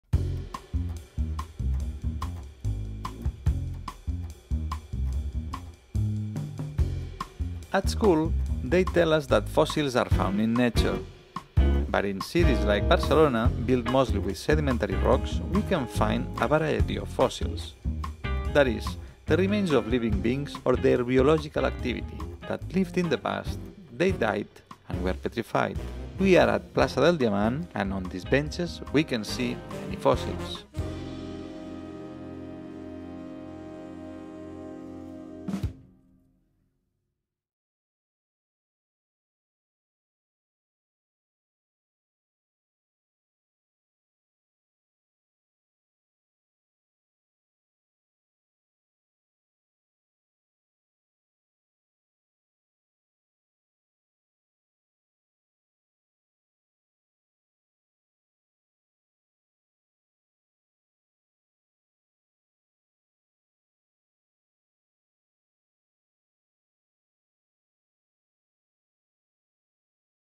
Augmented City: The history of the planet and its life
This video is one of the stops of Augmented City, a project by La Mandarina de Newton that presents Barcelona as a museum in itself. Full of interesting pieces that can be found in the most diverse corners: streets, squares, cultural spaces, research centers..., and also museums, of course.
Choose your route on ciutataugmentada.barcelona or visit ciutatculturaiciencia.barcelona.